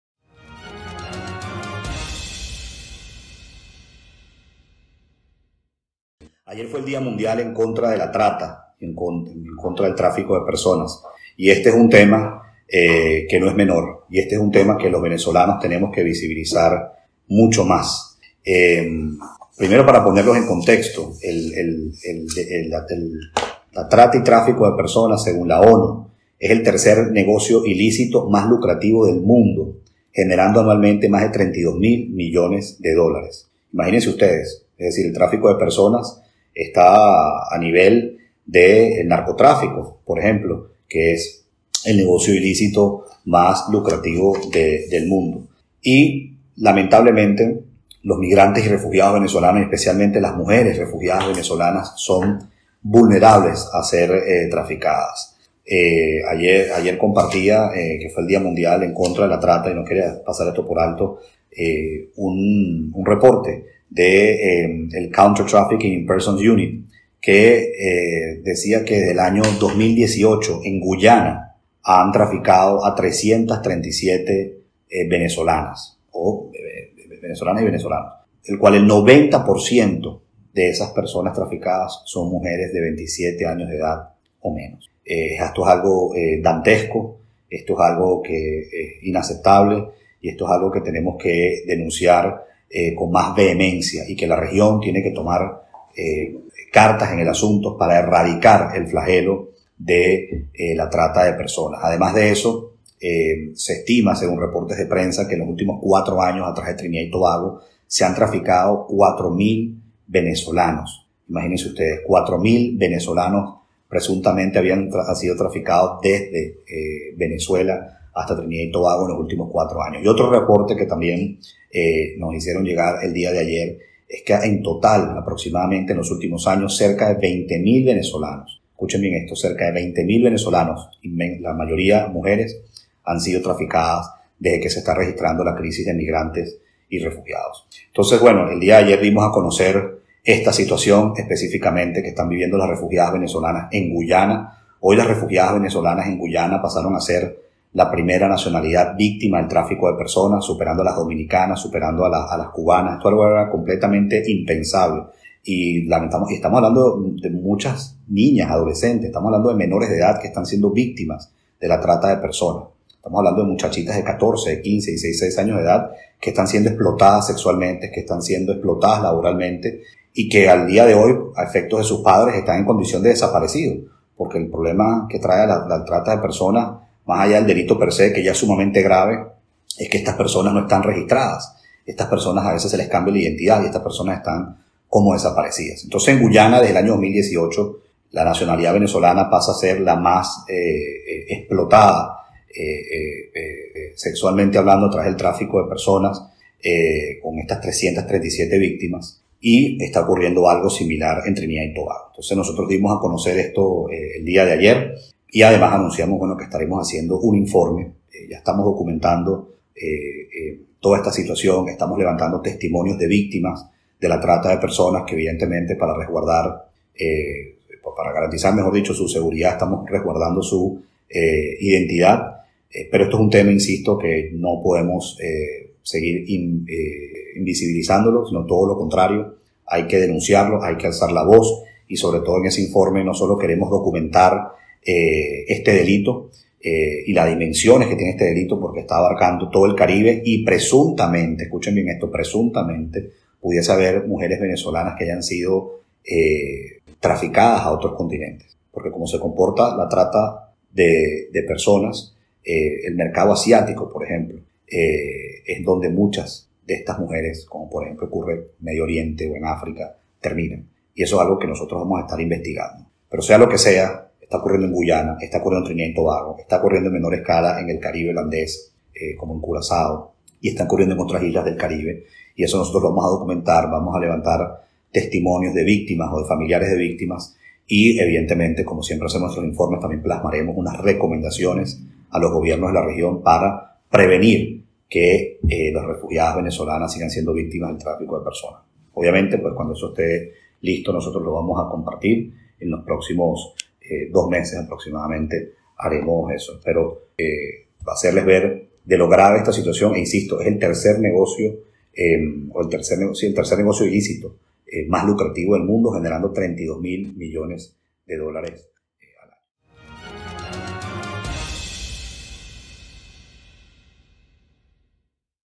DENUNCIA DAVID SMOLANSKY 1AGO ALGO INACEPTABLE PASA EN VENEZUELA
Smolansky: “Tenemos todo el drama que se da hacia el mar Caribe, sobre todo entre el estado Sucre y Trinidad y Tobago. Se calcula que al menos 4.000 venezolanos han sido traficados a través de Trinidad, muchos de ellos desesperados por salir de la crisis”.